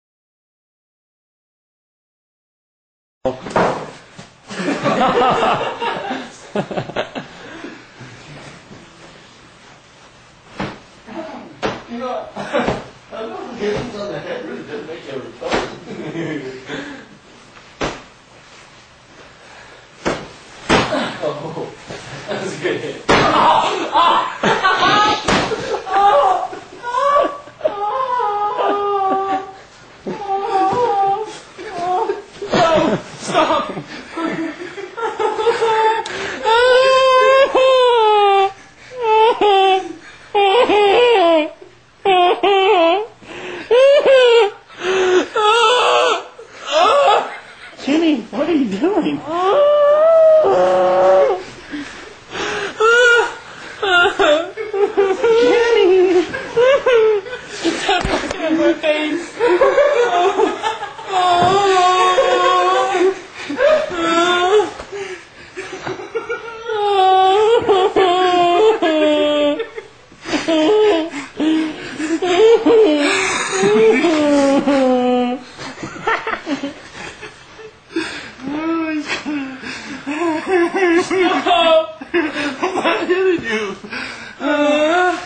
Pillow fight sissy
Grown man crys like a baby